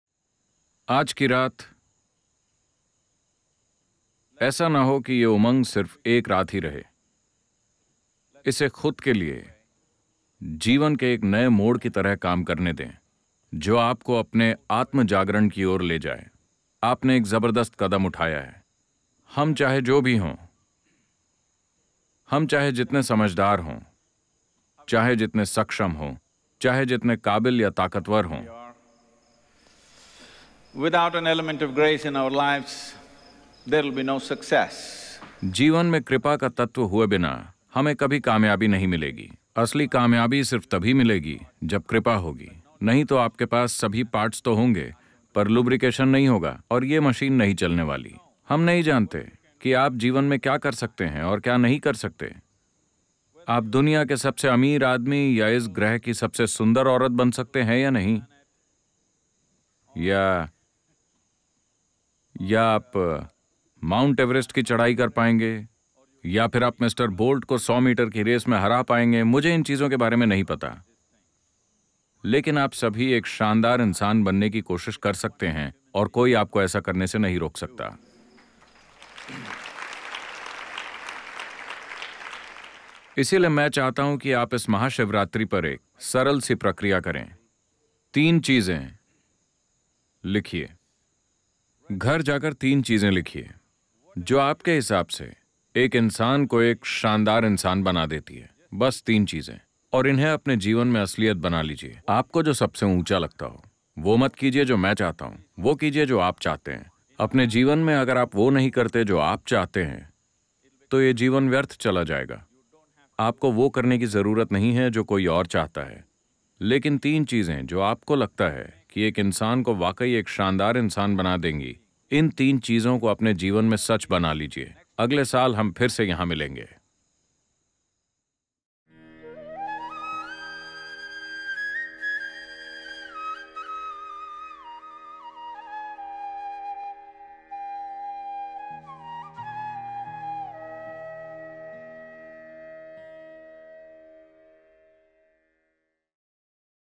तीन चीज़ें जो आपको एक शानदार इंसान बना देंगी। A Simple Process to Transform Yourself [Hindi Dub]
महाशिवरात्रि के अवसर पर सद्‌गुरु ने सभी से खुद को रूपांतरित करने का आह्वान किया और ऐसा करने का एक सरल तरीका भी बताया।